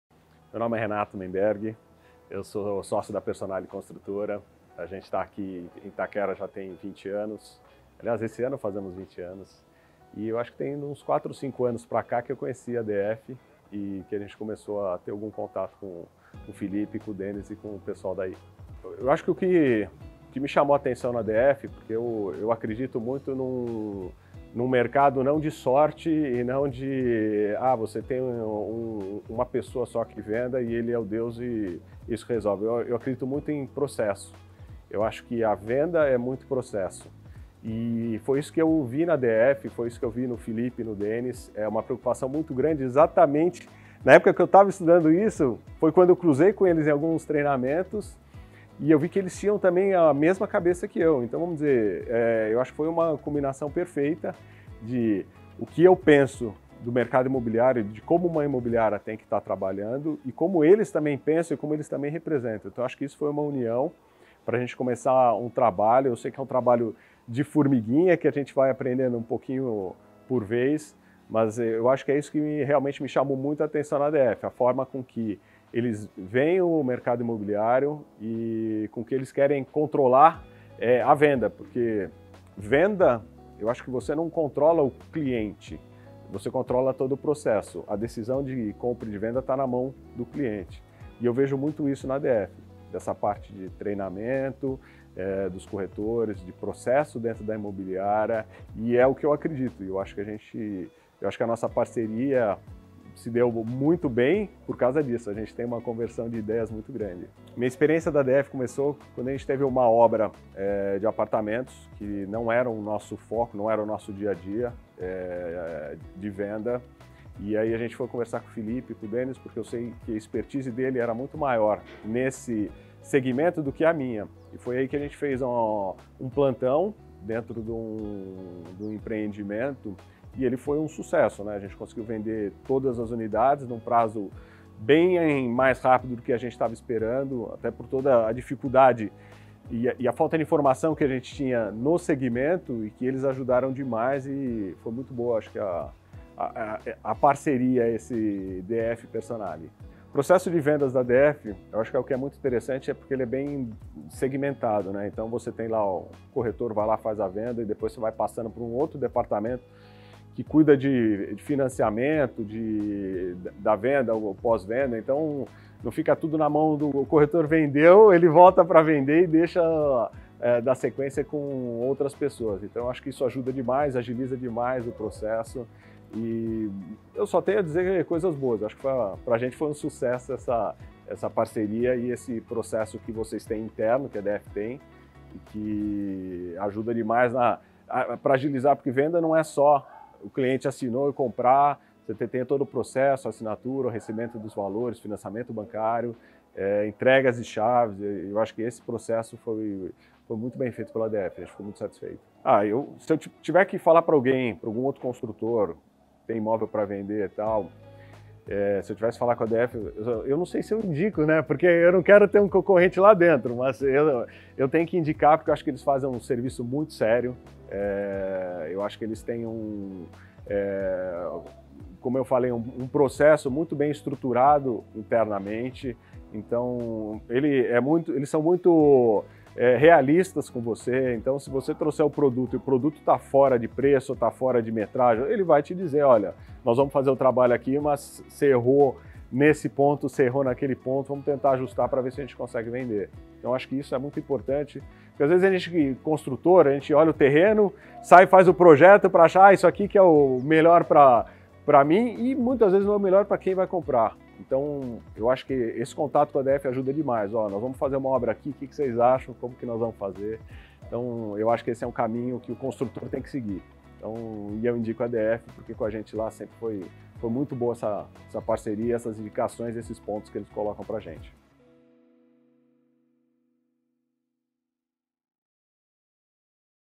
Construtor: A história de Renato
Saiba mais sobre a história do Renato, sócio da Construtora Personale, que deixou os imóveis com a DF Casa Imóveis e veio contar como foi a sua experiência.

Imobiliária em Itaquera

CRECI: J-25369